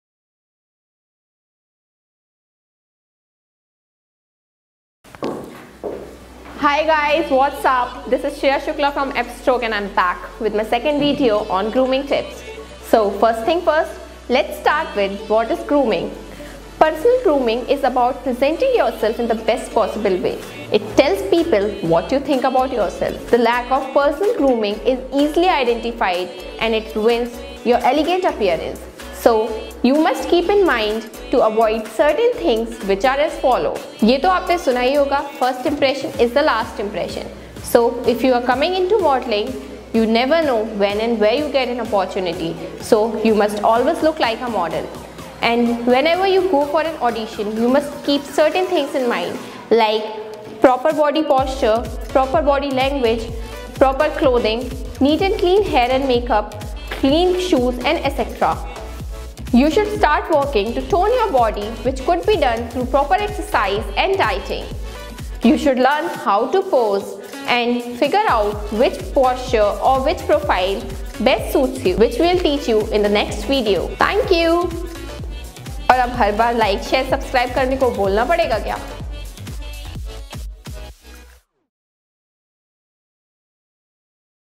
Grooming Tips for Models | Modelling Tips Part-2
Hi Guys!
This video is for those who wants to start his/her career in modelling. In this video we have described about grooming needed for models.